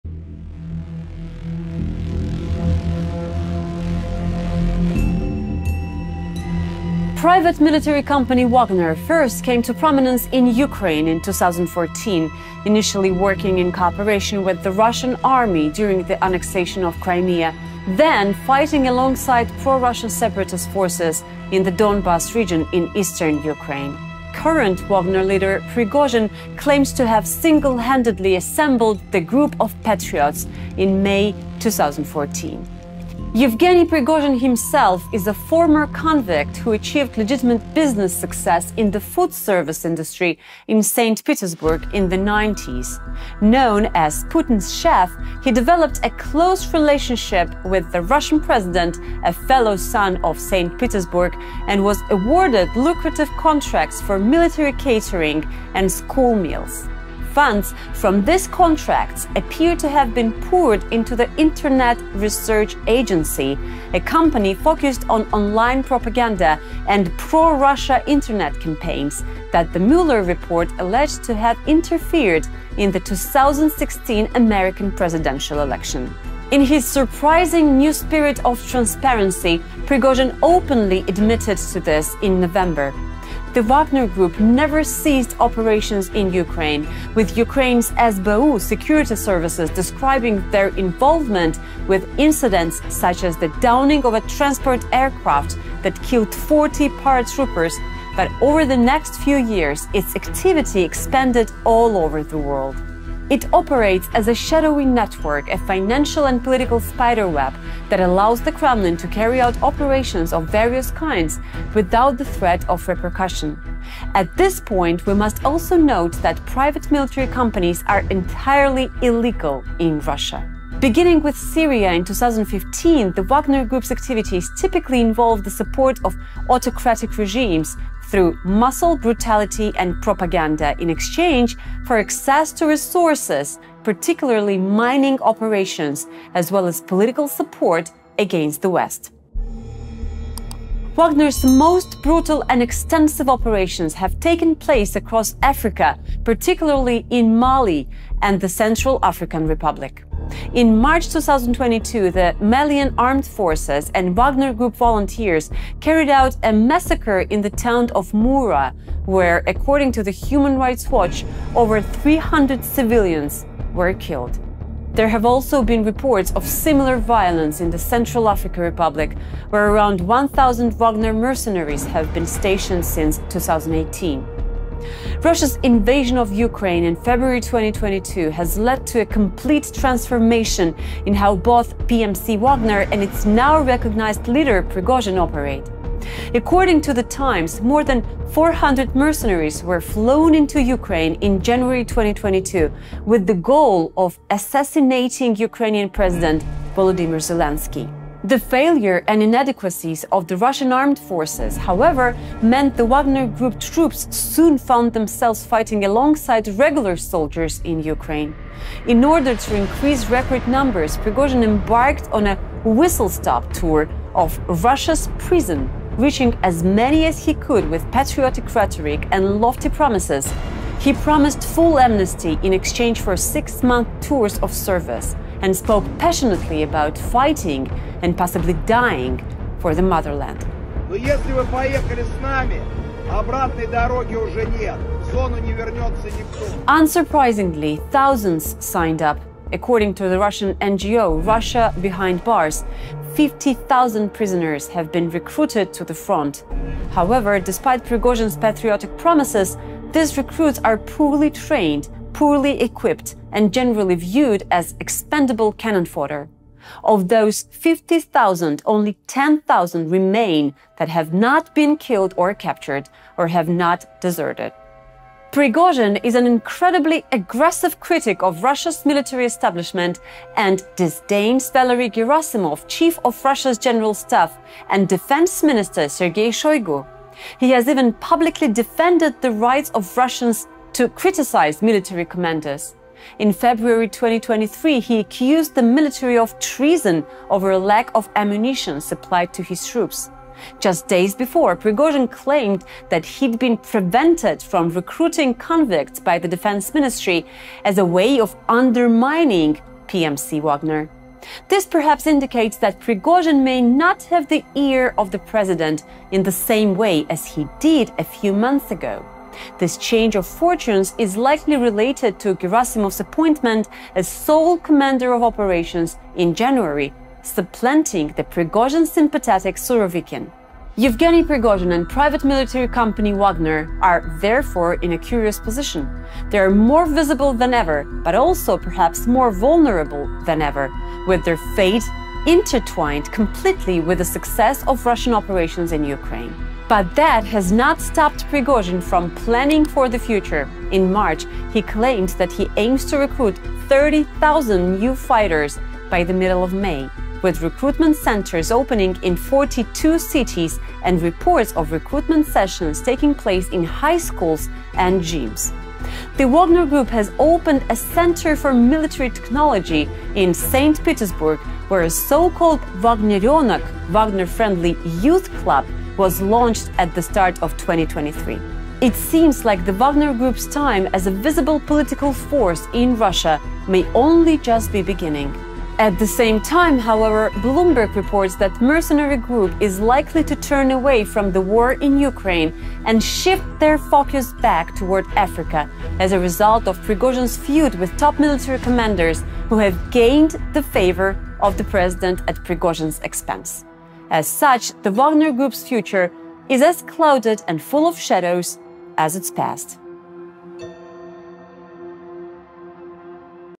The Story of Yevgeny Prigozhin and The Wagner Group
Evgeny Prigozhin and his PMC Wagner troops have allegedly launched a rebellion against the Russian armed forces and are marching on Moscow. But who is Prigozhin, and how did it come to this? Watch our explainer to find out.